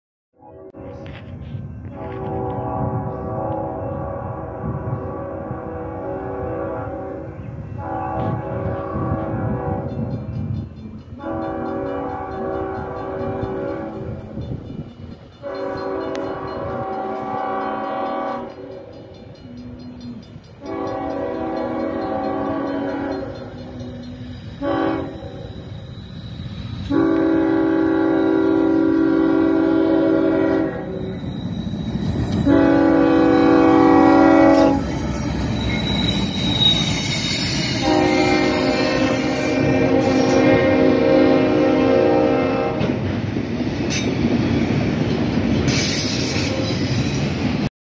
KCSM 4565 Trails on M206 through Terre Haute, IN
UP #5503 C45ACCTE (Flags) 3rd Gen K5HL
KCSM #4565 AC44CW (Grey Ghost)